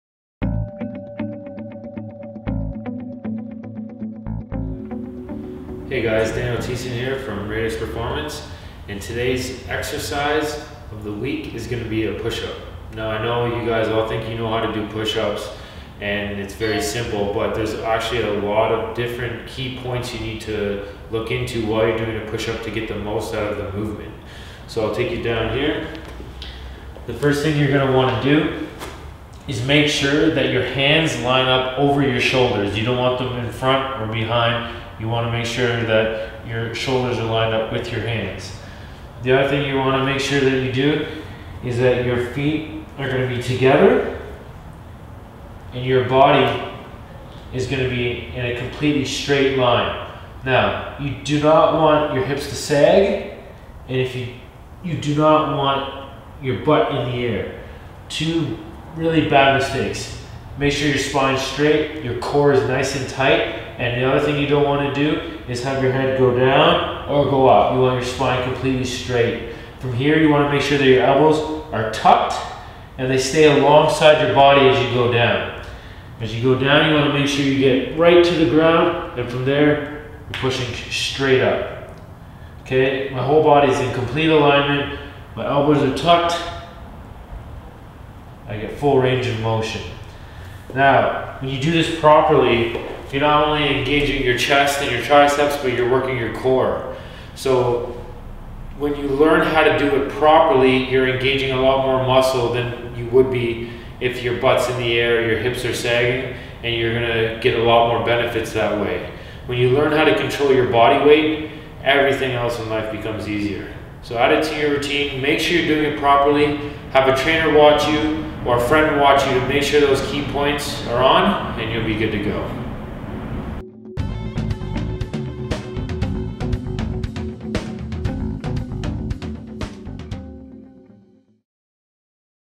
Pushups - How To - Radix Performance
Learn how to perform pushups with the new exercise and stretch video series from Radix Performance.